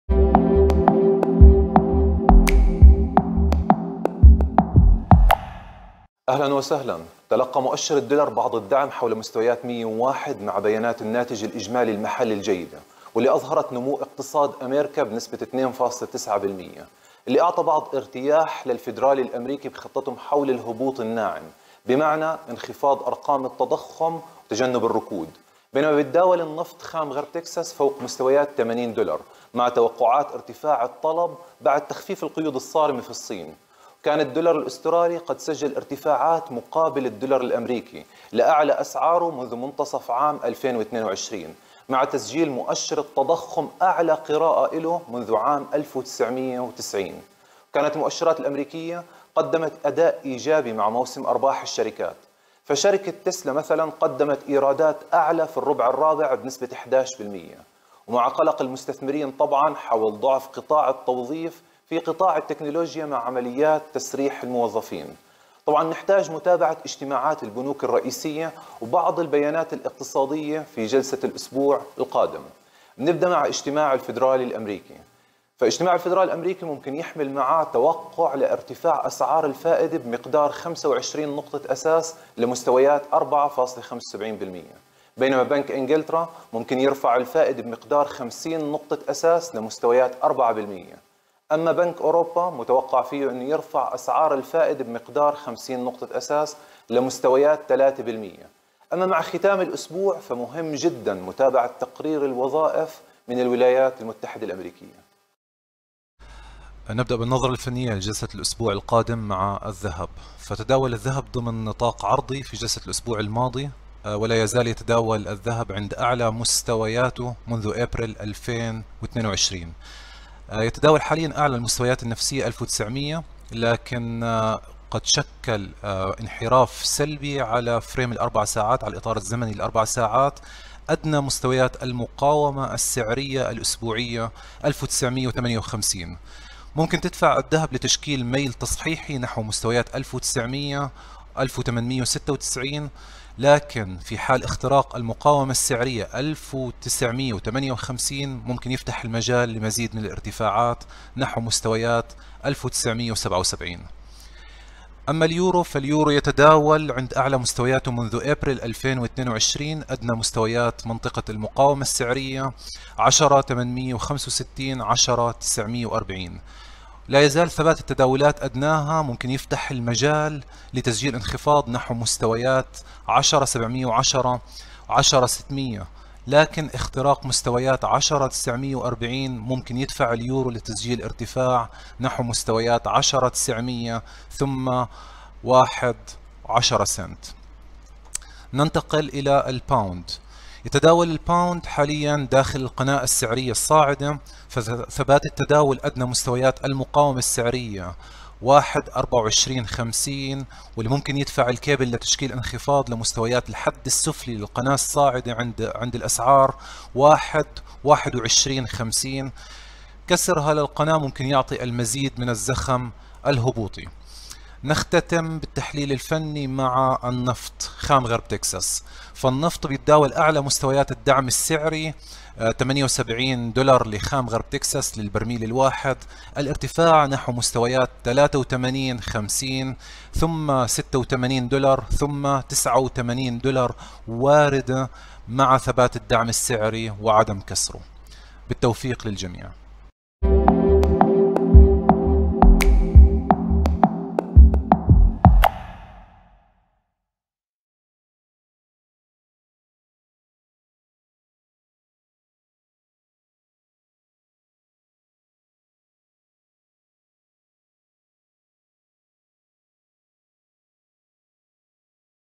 نظرة أسبوعية على أسواق المال - اجتماعات البنوك هي الأهم لتحركات الدولار واليورو والباوند
ماذا تترقب الأسواق هذا الأسبوع؟